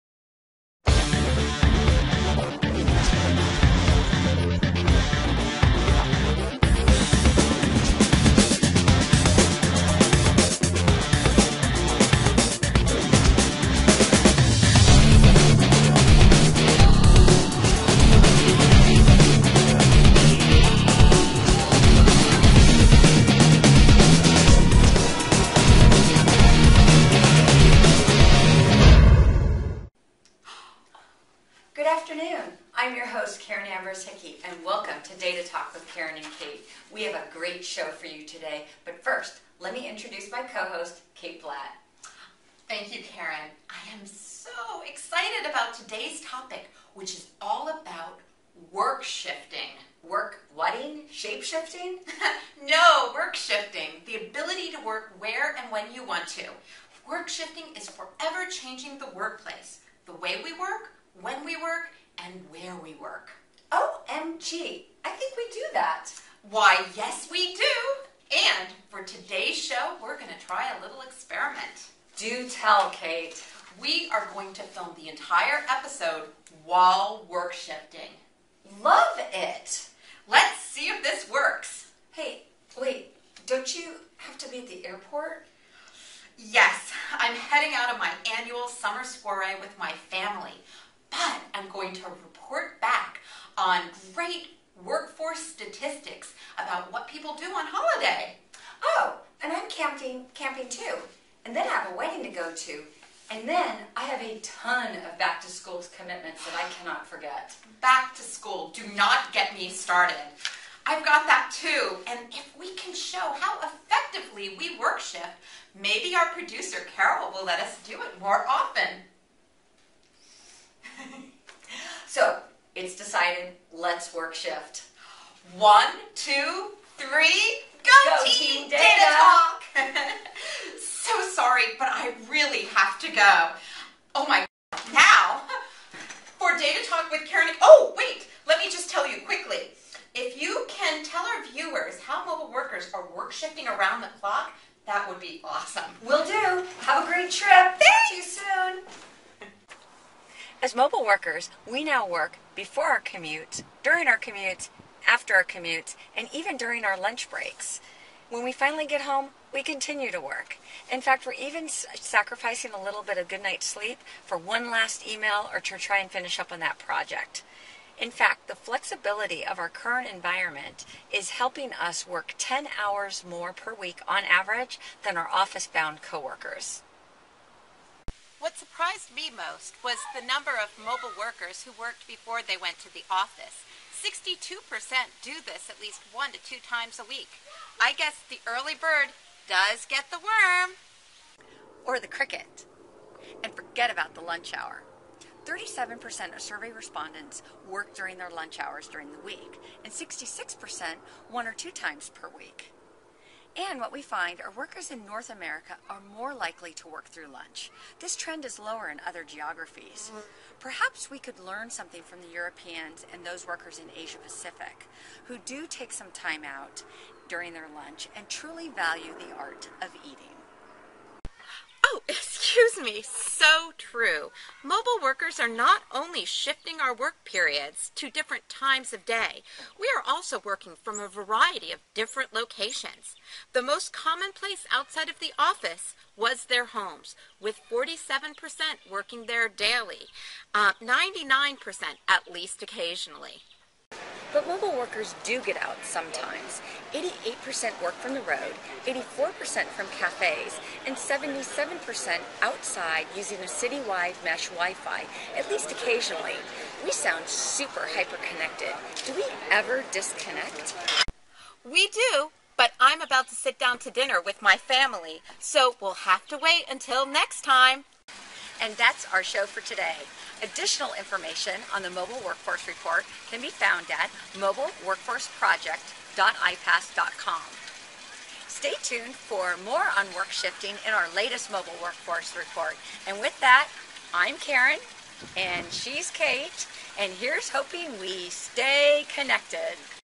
Mobile Employees Work Around the Clock According to New iPass Mobile Workforce Report
Mobile Workforce Report Data Talk Episode on Workshifting